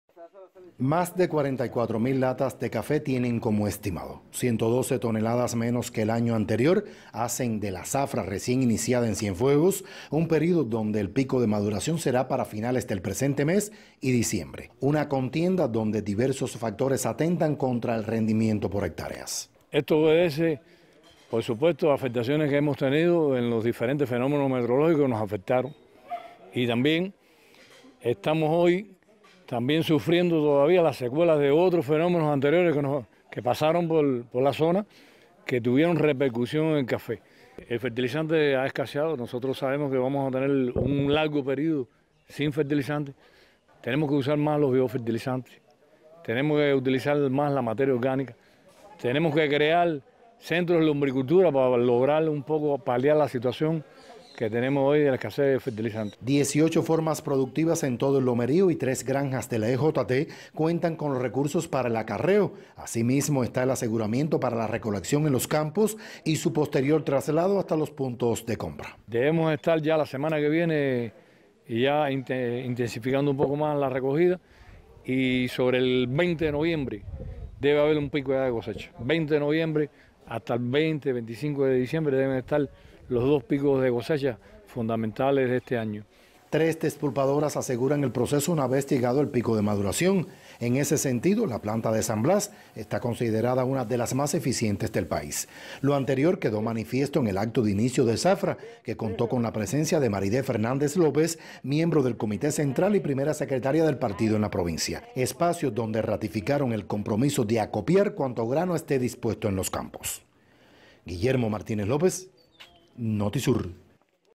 Disminuye estimado del café en la recién iniciada cosecha del grano en Cienfuegos
Con un estimado inferior a la pasada contienda inició en Cienfuegos la zafra del café. Daños en las plantaciones unido al déficit de fertilizantes propicia que disminuyan los rendimientos por hectáreas, aun así garantizan los compromisos para la exportación desde la empresa Eladio Machín de Cumanayagua.